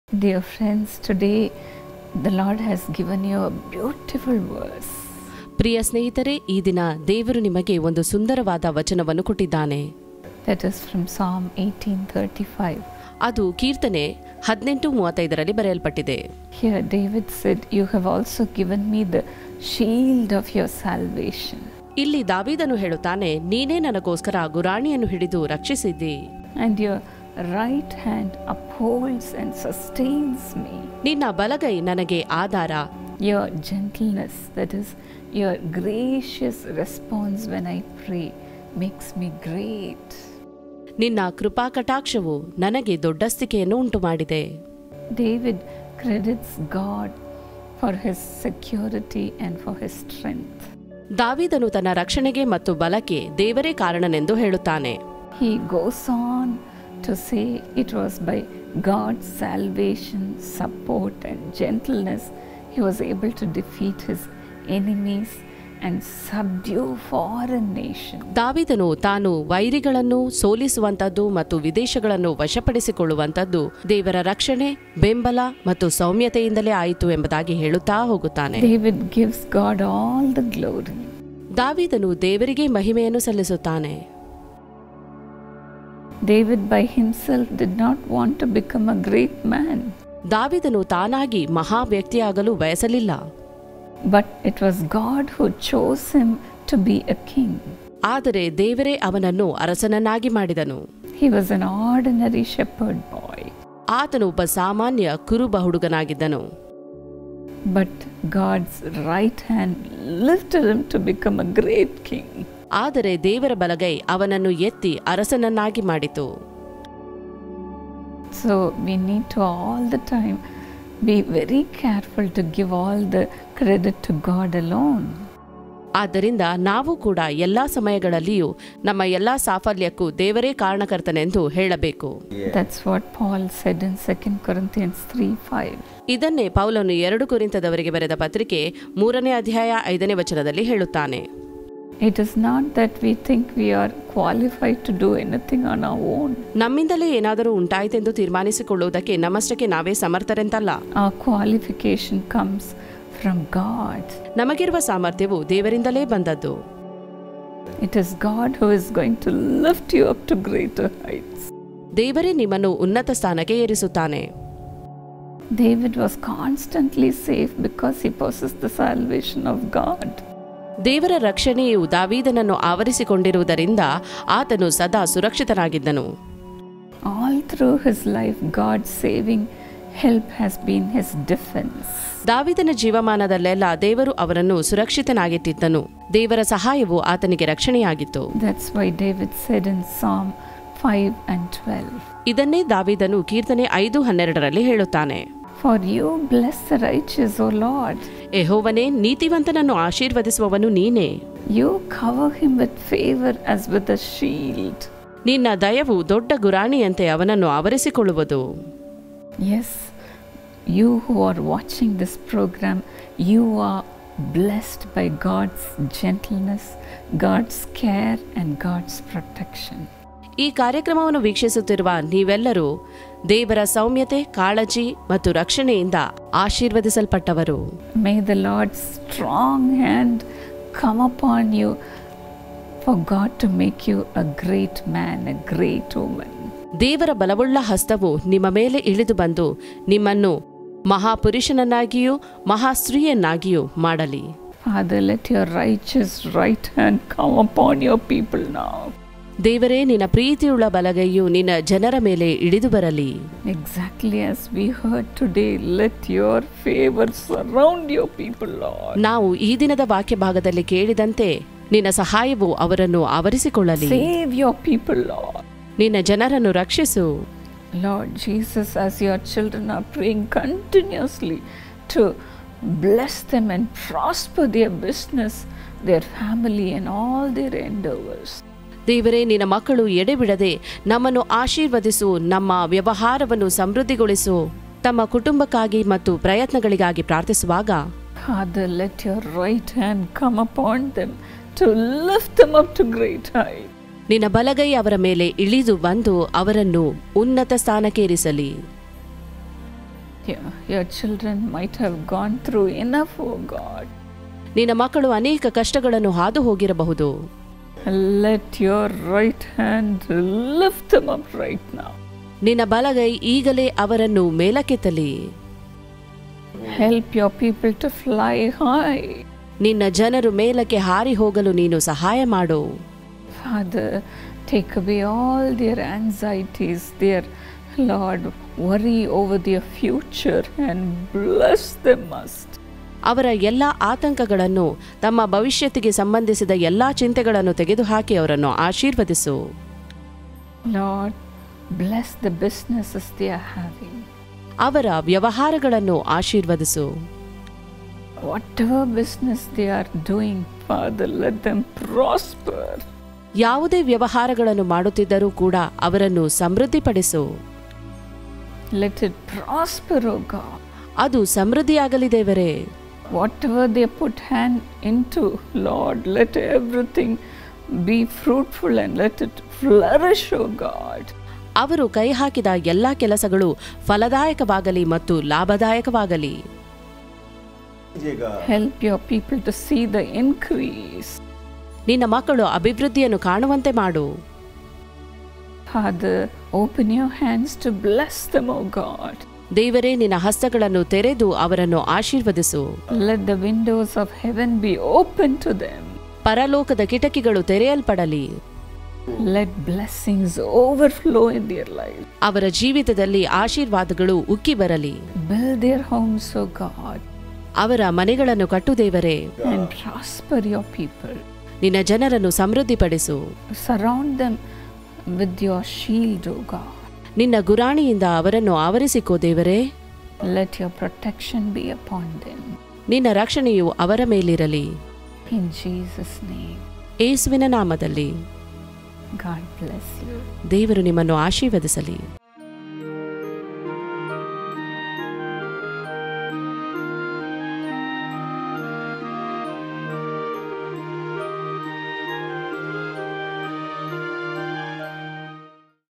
Salvation - Free gift of God | Sis. Evangeline Paul Dhinakaran | Today's Blessing
Your sufficiency and qualification come from God! You are indeed blessed by God’s gentleness, care and protection. His mighty hand will make you great. Tune in to this blessing by watching today's promise.